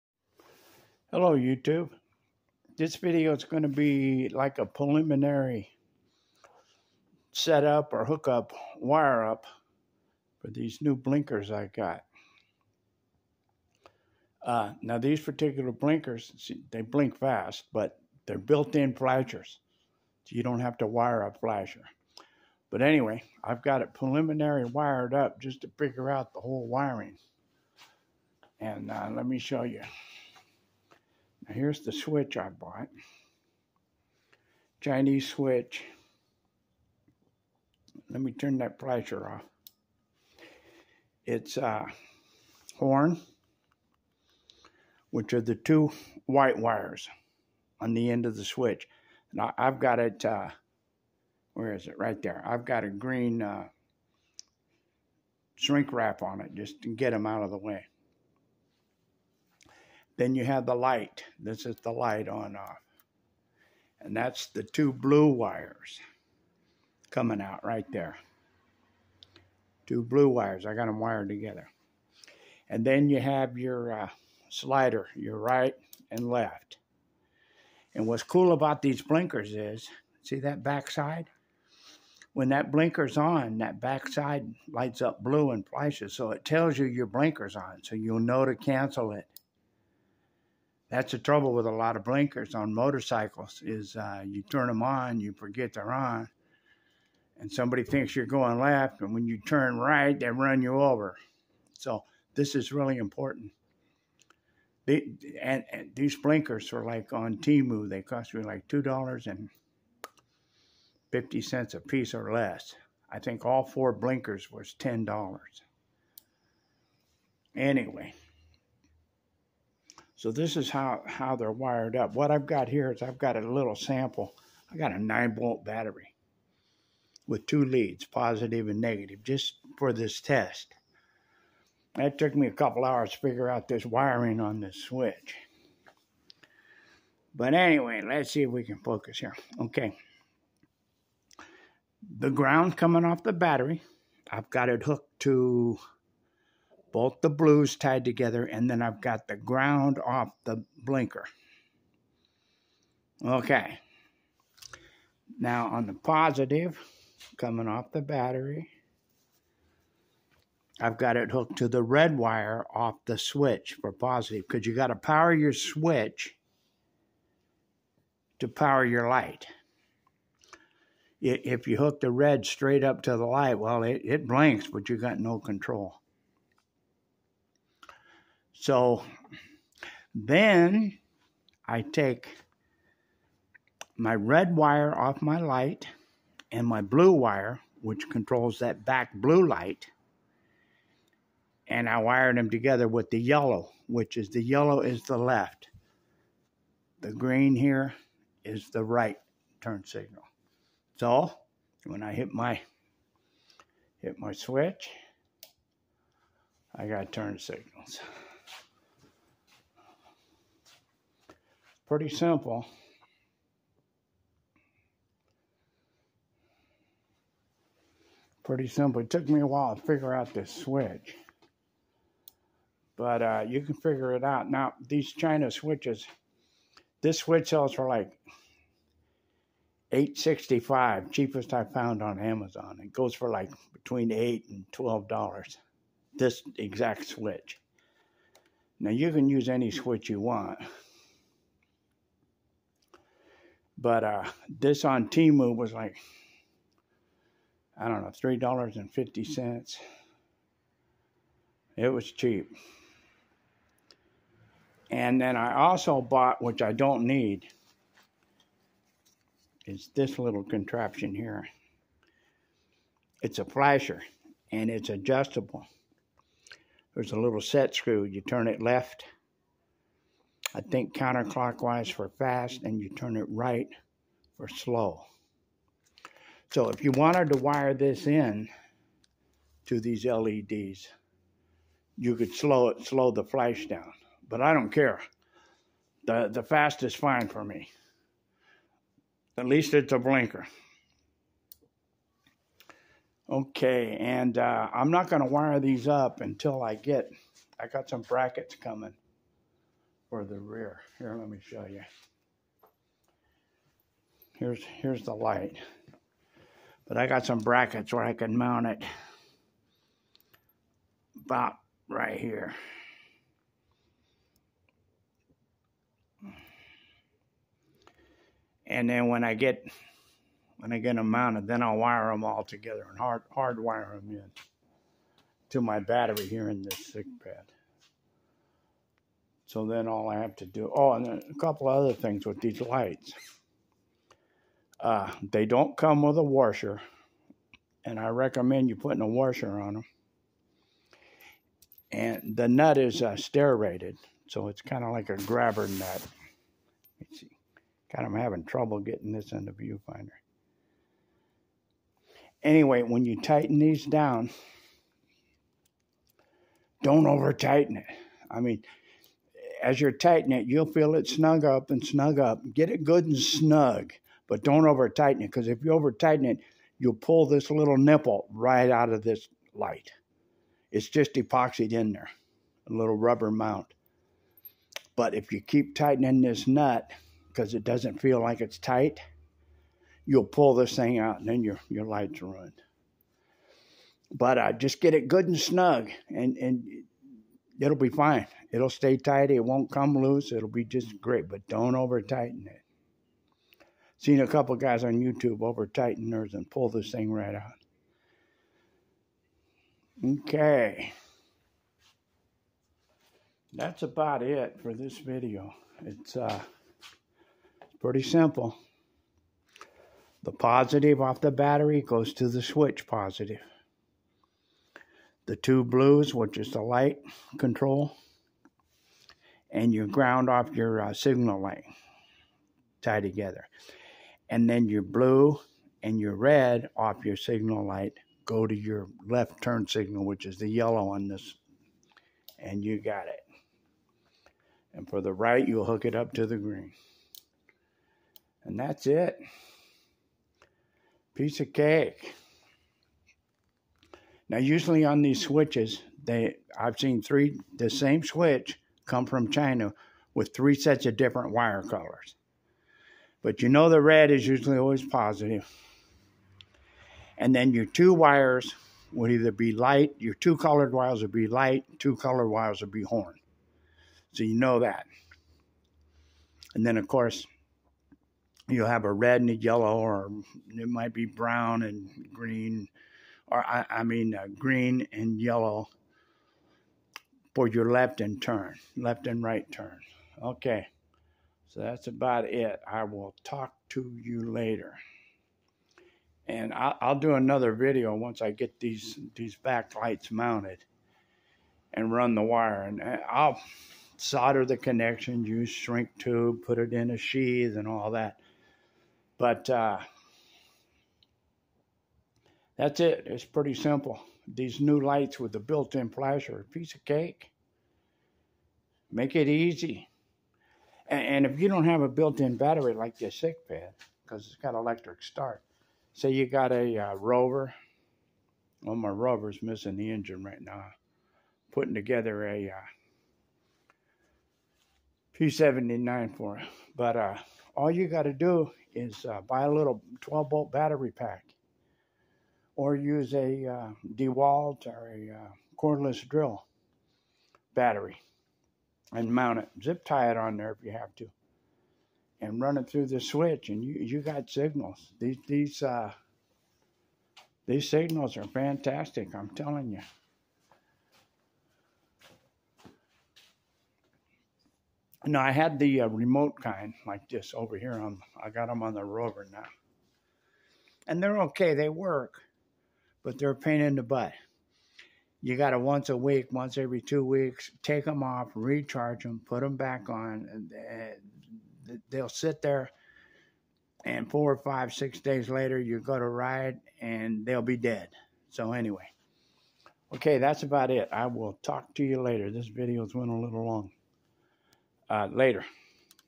Phatmoto/Sikk Ped : LED sequential blinkers testing
A quick pre wiring of LED turn signals. Figuring out the switch wiring before hard wire in.                        Colors for this switch : Red = + from battery, 2 Blue = headlight, 2 White = horn, Yellow = left turn signal, Green = right turn signal. Colors from the turn signal light :  Red = positive connect to left Yellow or right Green at switch, Blue = positive connect to left Yellow or right Green at switch, Black = - Negative/Ground to connect to battery ground and 2 Blue wires from switch.